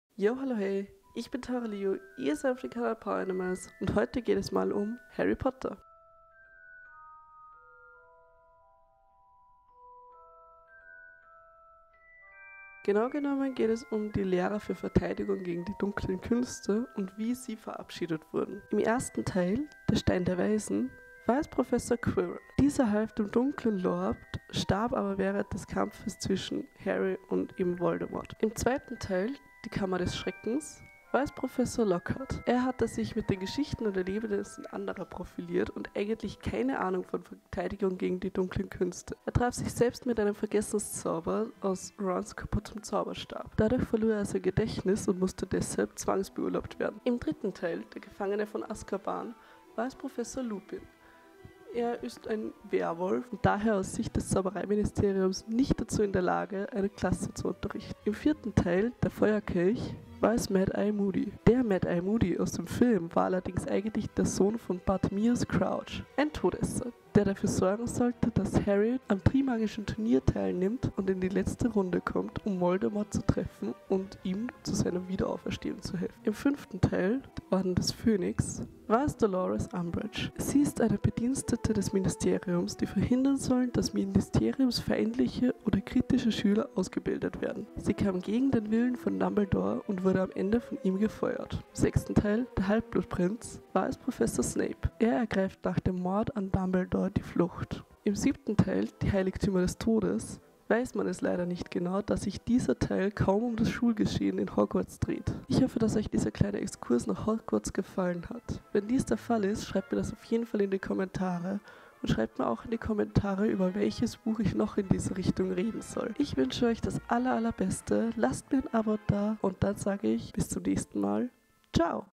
Lehrer für Verteidigung gegen die dunklen Künste - Harry Potter
Hey!
in diesem Video klären wir einmal auf, welche Lehrer bereits das Amt als Lehrer für Verteidigung gegen die dunklen Künste in den 7 Teilen von Harry Potter innehatten.

Alle Rechte an verwendeter Musik gehen an die Urheber!